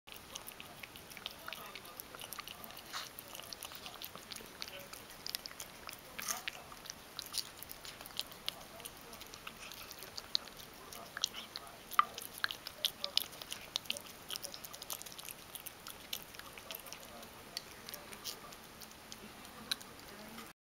【猫のもじお君（ラグドール）】 食事中③ 涙を流しながら餌を食べる 【Ragdoll cat to eat the food in tears】
9秒の時、涙がこぼれます。
エサを食べていると、ときどき涙を流しながら食べます。病気じゃないか少し心配ですが、美味しくて泣いているのかな？
As I ate, I eat in tears from time to time. It is a little worried or not you sick, how are you crying and delicious?
正如我吃，我吃的眼泪不时。这是一个有点担心，或不是你生病了，你怎么哭了又美味？